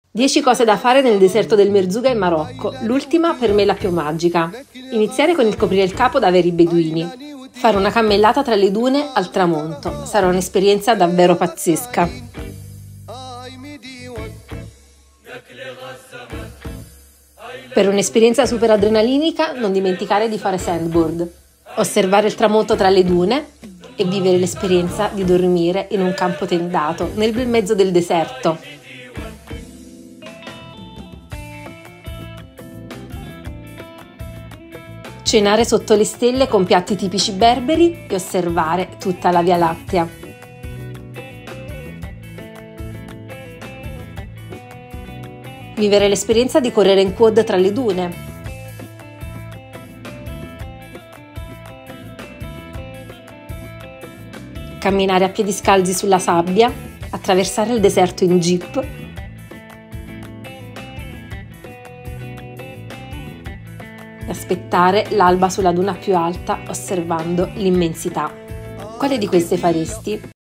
10 cose da fare nel deserto del Merzouga in Marocco
10 cose da fare nel deserto del Merzouga in Marocco 🇲🇦
L’ultima è la più magica!

1️⃣ Coprire il capo da veri beduini
2️⃣ Fare una cammellata tra le dune al tramonto
3️⃣ Fare sandboard, super adrenalinico
4️⃣ Osservare il tramonto tra le dune
5️⃣ Dormire in un campo tendato nel bel mezzo del deserto
6️⃣ Cenare sotto le stelle con piatti tipici berberi e osservare tutta la Via Lattea;
7️⃣ Correre in quad tra le dune
8️⃣ Camminare a piedi scalzi sulla sabbia
9️⃣ Attraversare il deserto in jeep
🔟 Aspettare l’alba sulla duna più alta osservando l’immensità!

Quale di questa faresti?